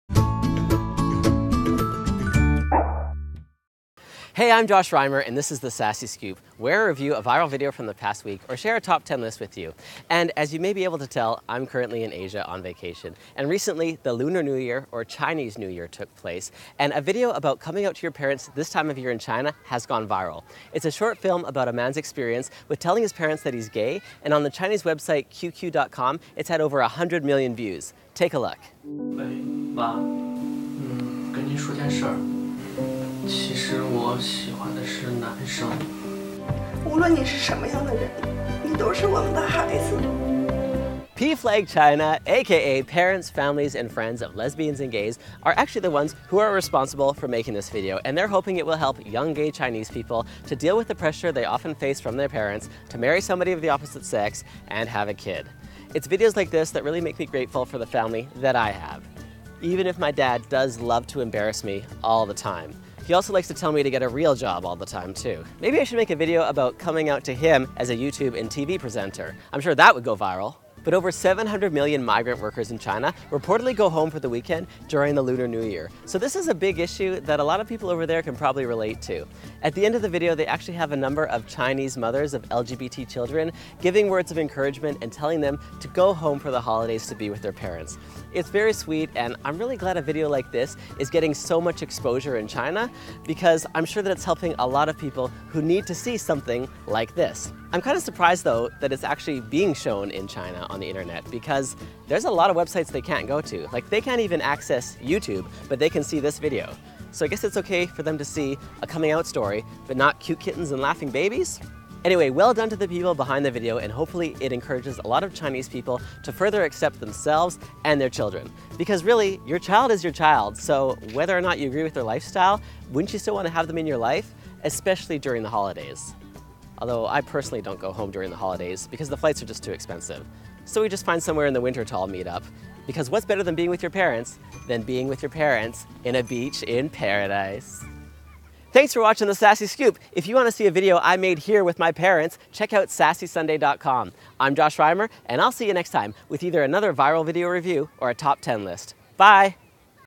The Sassy Scoop: Chinese New Year (Viral Video Review)
In China it's an important tradition for people to go home to see their parents during the Lunar New Year. For the LGBT community this can be a difficult things to do so PFLAG China created a short film to encourage them and it's gone viral, receiving over 100 million views. and follow @JoshRimer.com!